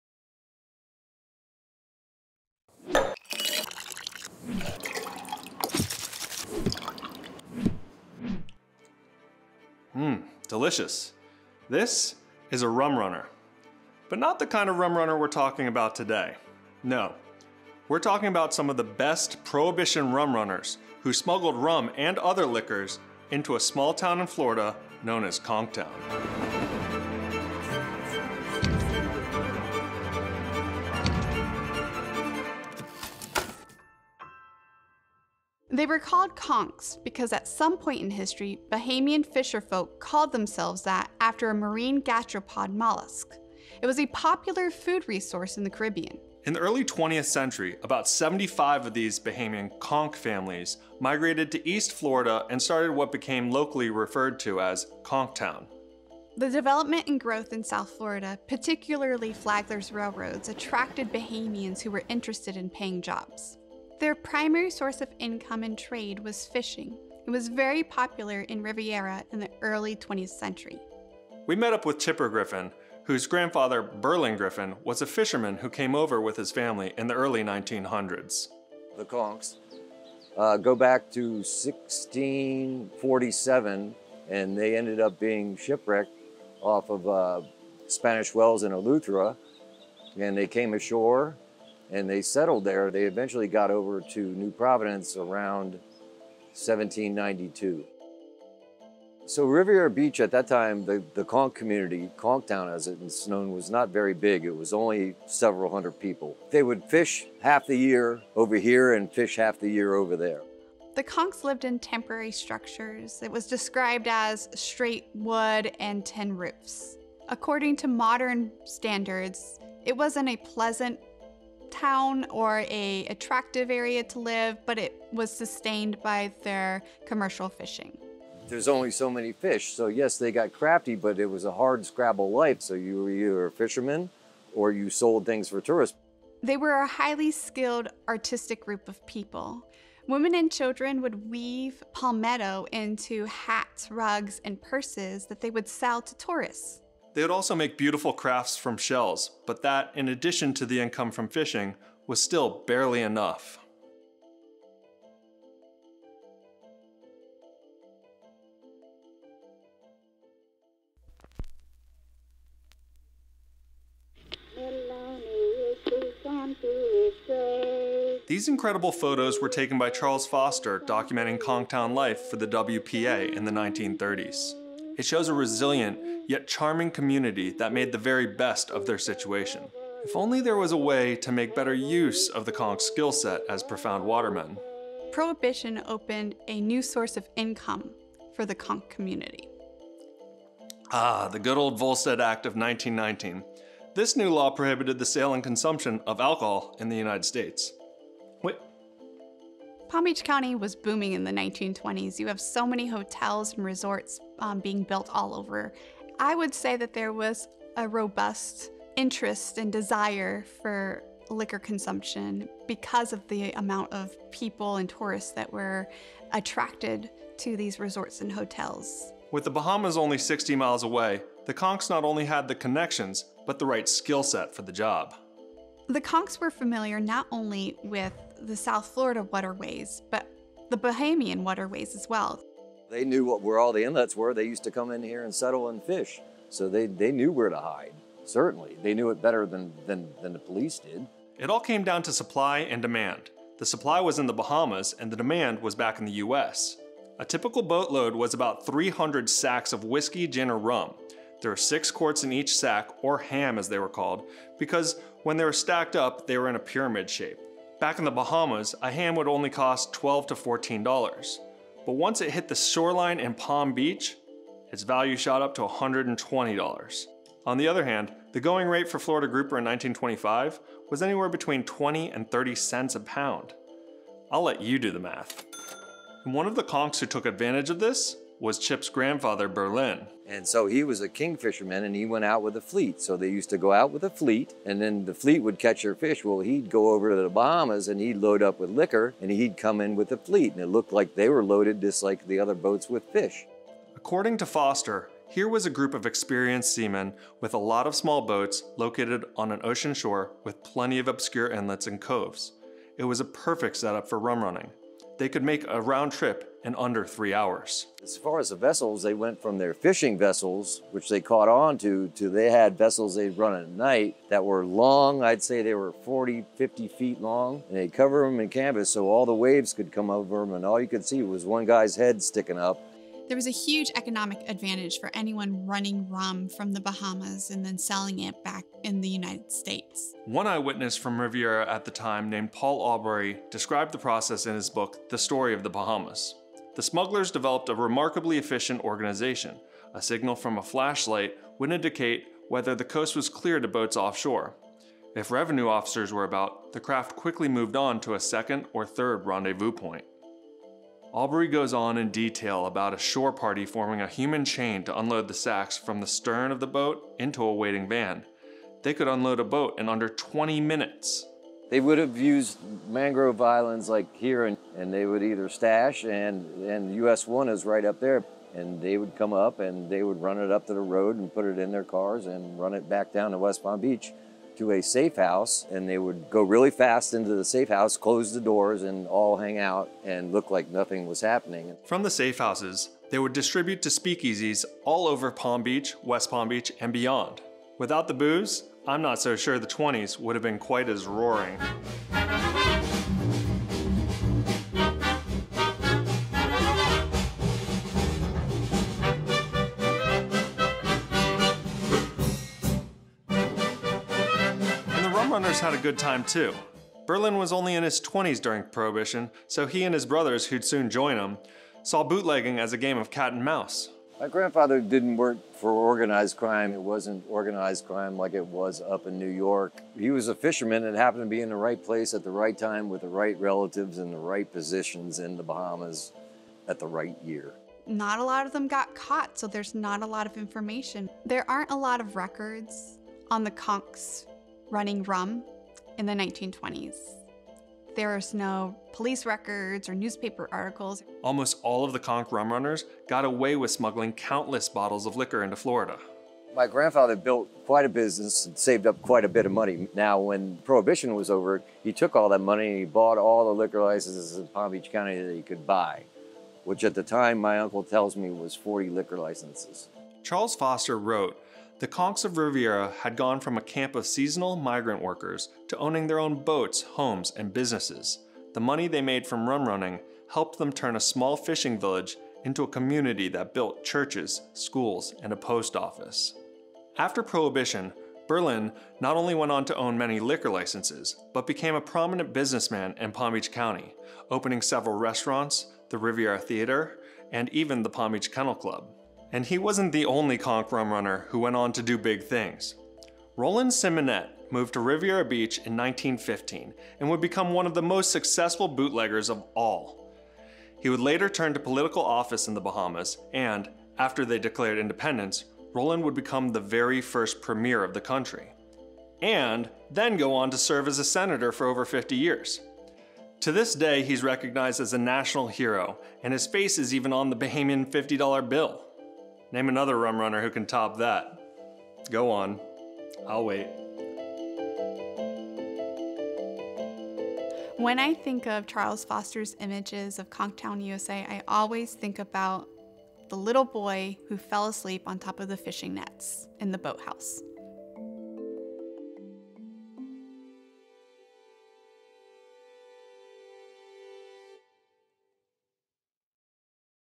The Conchtown Rumrunners - Behind the Palms. Episode 2
The Historical Society of Palm Beach County (HSPBC) is proud to release a new episode in its video series Behind the Palms, hosted by Kent Anderson and produced by KiloContent.

Behind the Palms breathes life into the homegrown history of Palm Beach County and the exciting details of never-before-heard stories. Episode 2, “The Conchtown Rumrunners,” dives into the remarkable history of original Conchtown residents and their connections to illicit rumrunning during Prohibition.